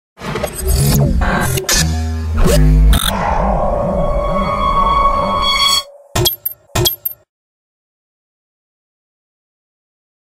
Logo animation - Position Edge
This logo sting was completed for Position Edge, an internet marketing company. It was produced using After Effects and will be used at the beginning and end of each of their videos.

video produced by MotionEcho Media